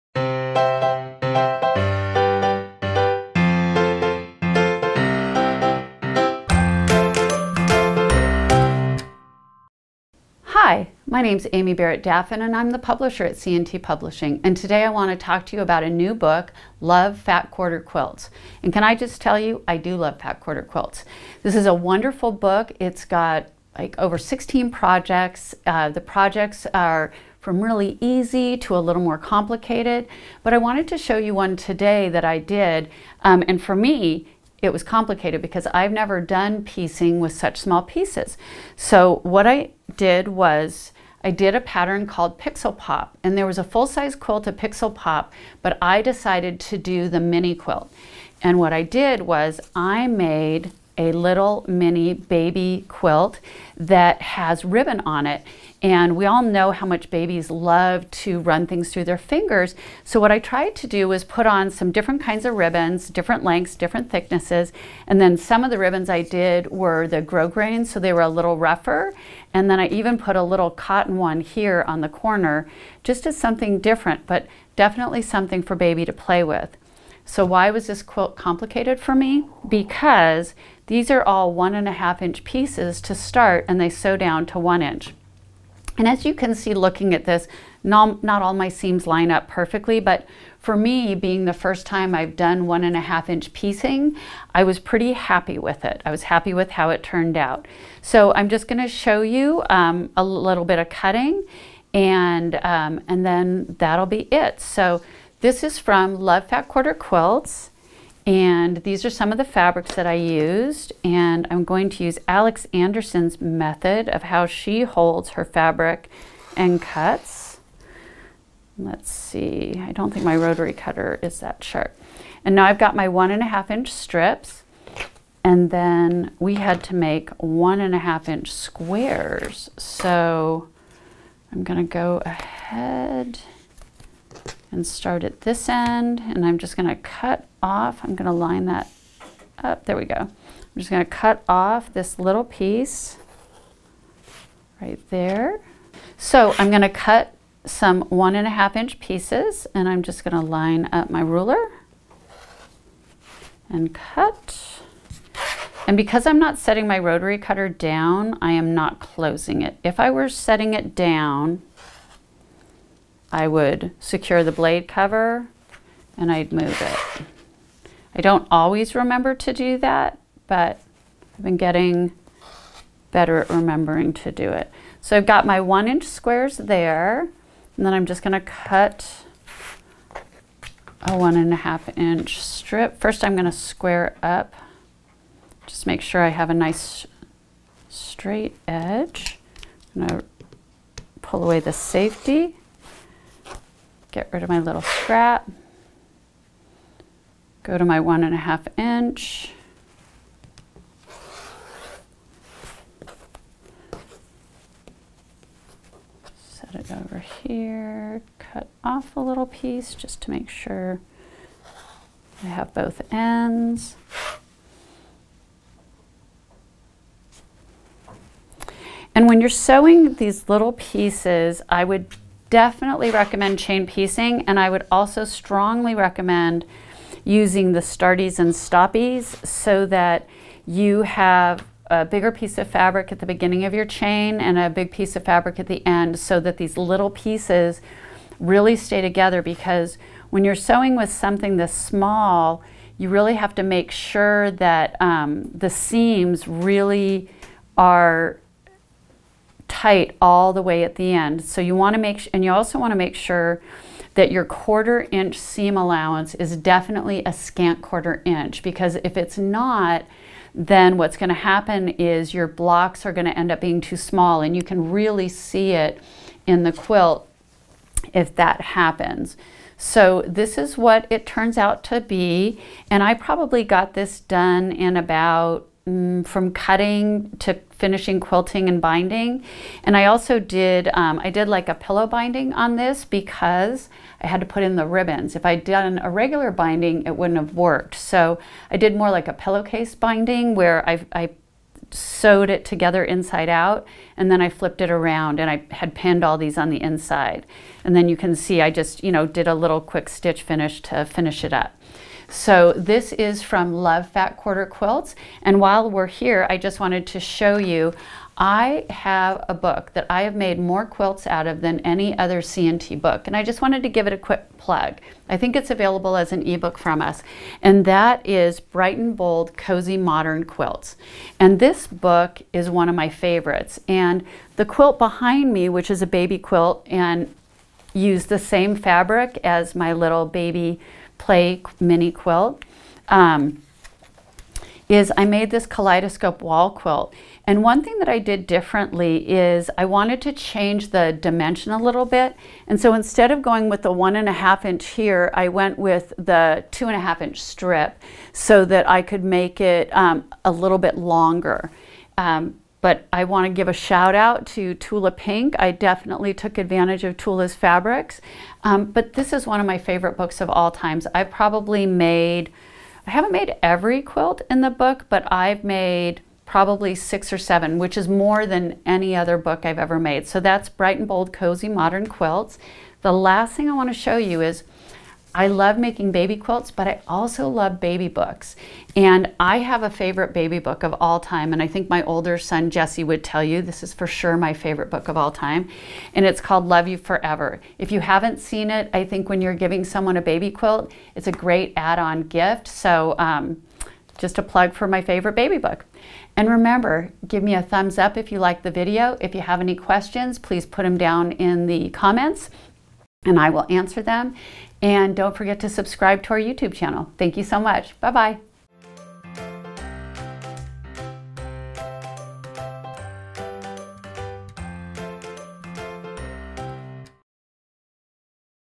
How to Make a Baby Quilt
Watch C&T Publisher Amy Barrett-Daffin use the Pixel Pop pattern from Love Fat Quarter Quilts to create a baby quilt. She gives out helpful quilting tips for piecing with small pieces.

Love Fat Quarter Quilts has a colorful and eye-catching collection of 20 projects using fat quarters ranging from bed-size quilts to cushions and smaller projects.